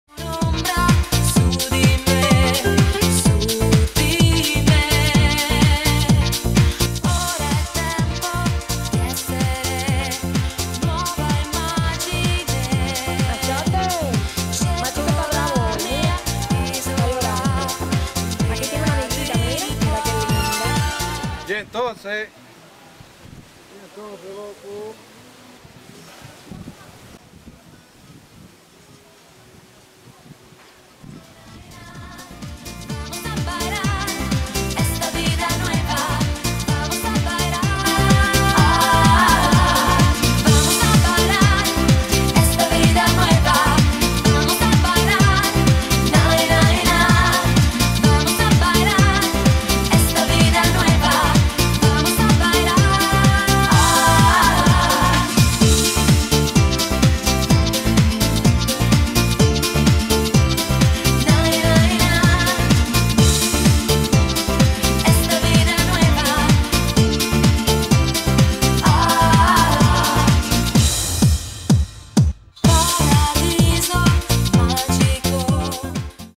Hotel Coral Marien Puerto Plata JUNIO 2002 Be Live Grand Marien 10
Hotel Coral Marien Puerto Plata JUNIO 2002 hoy dia 2012 llamado  Be Live Grand Marien 8.Esto es un gran recuerdo para aquellos que viajaron a este hotel.En YOUTUBE NO HAY MUCHOS VIDEOS DE ESTE HOTEL CUANDO ERA Coral Marien.